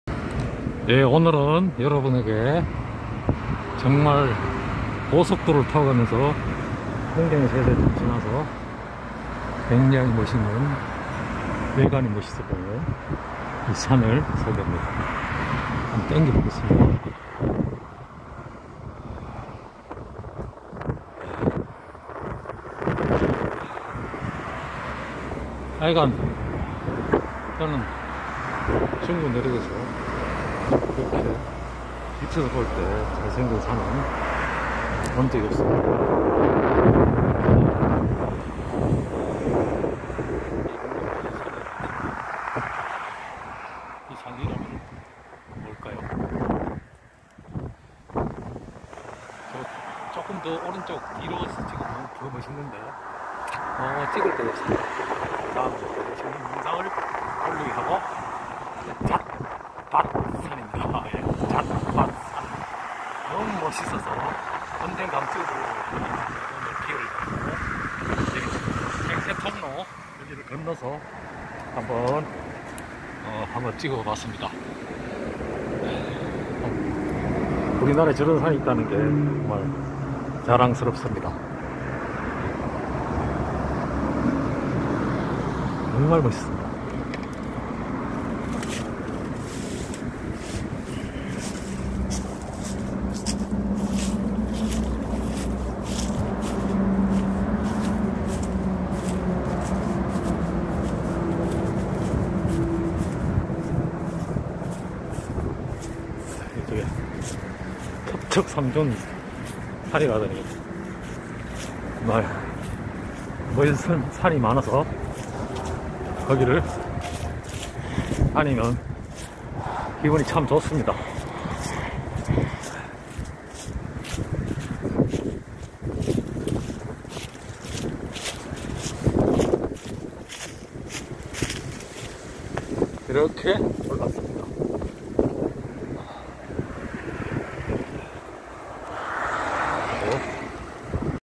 붕어섬 54 Carp island ㅡ 잣밭산, Mt. Jadbat in Moonkyoung city
문경새재 가까이 있는 산으로
여러번 고속도로에서 봤는데 사진을 찍지 못하였다.
자연생태통로에 올라가 촬영해서
중부 내륙고속도로에 있는 문경새재 휴게소 지나면 우측으로 나타 나는데
자태가 매우 웅장하며 이국적이다
다음에 기회가 되면 제대로 촬영할 예정이다